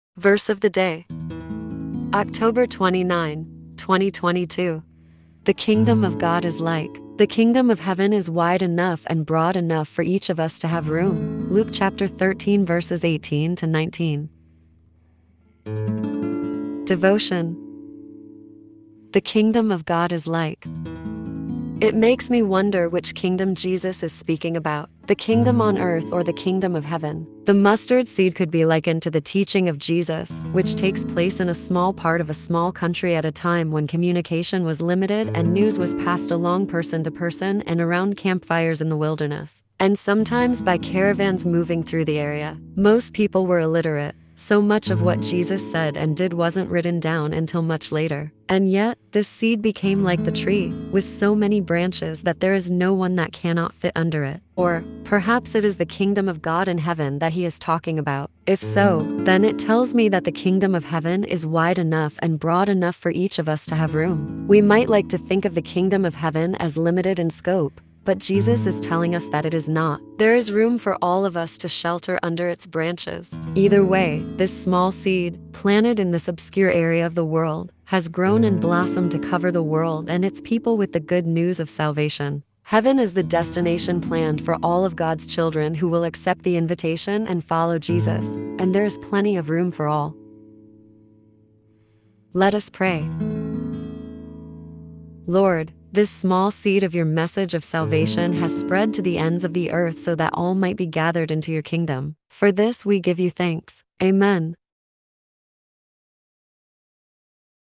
BIBLE VERSE OF THE DAY -October 29, 2022 | Devotion | Your Daily Dose of God's Word.
The Kingdom of God is like...
The kingdom of heaven is wide enough and broad enough for each of us to have room.
-Luke 13:18-19

Prayer:
Lord, this small seed of your message of salvation has spread to the ends of the earth so that all might be gathered into your kingdom. For this we give you thanks. Amen.